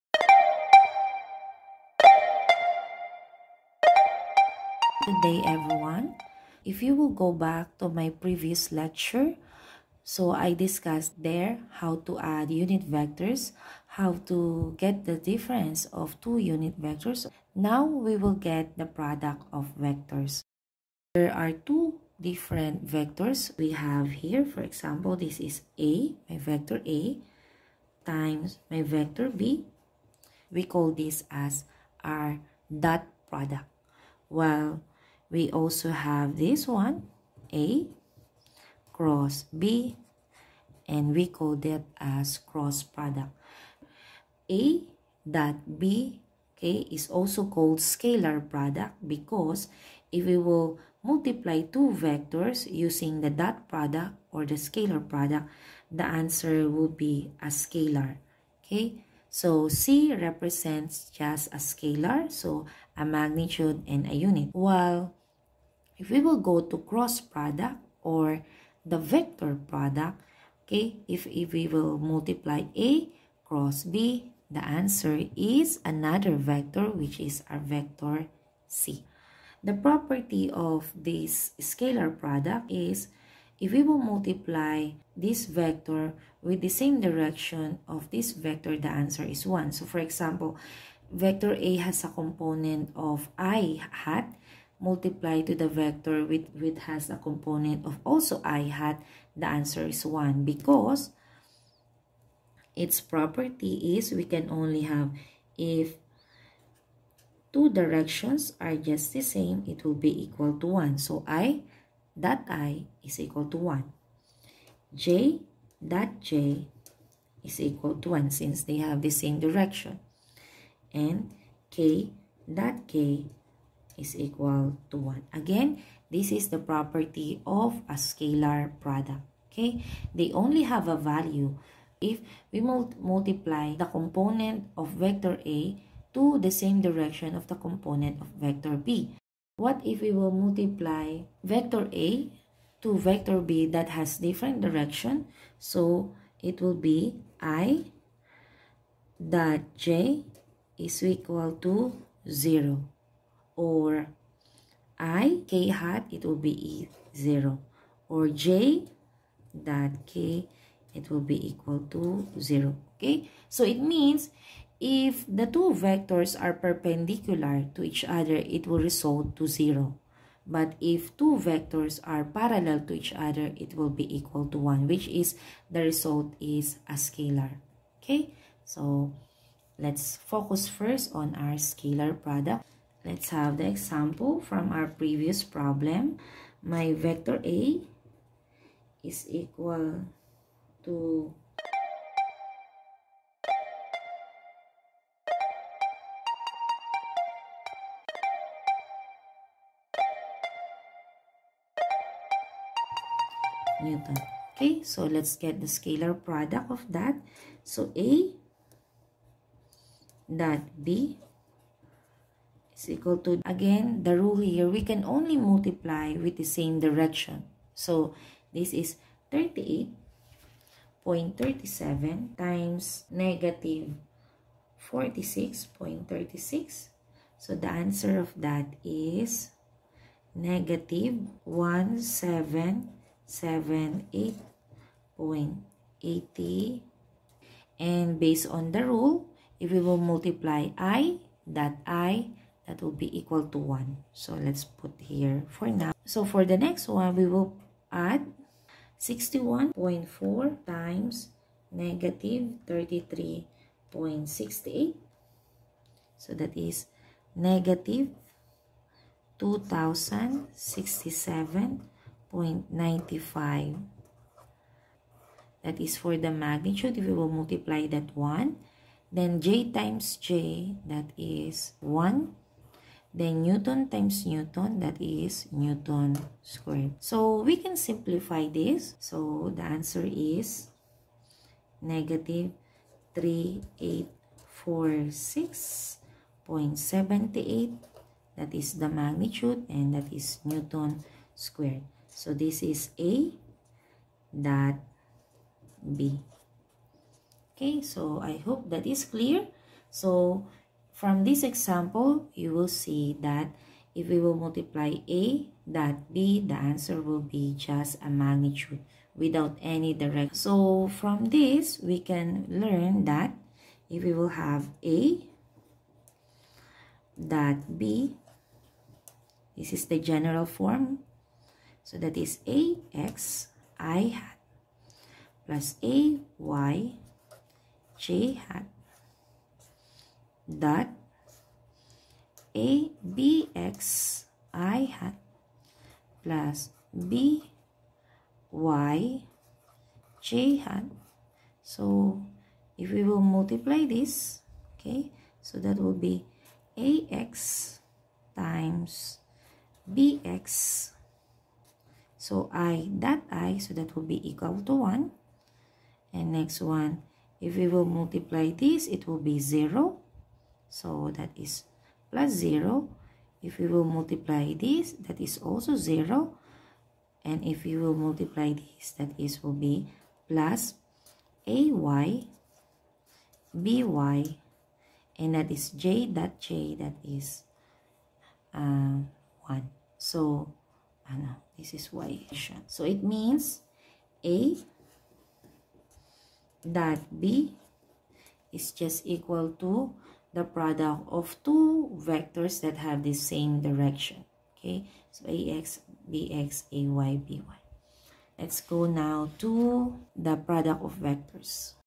Physics Lecture #2 (Part 3.1): Product of Vectors (Dot Product)
In this video, I explained:
(1) what is Scalar Product or Dot Product,
(2) how to determine the product of two vectors using the Scalar Product or Dot Product.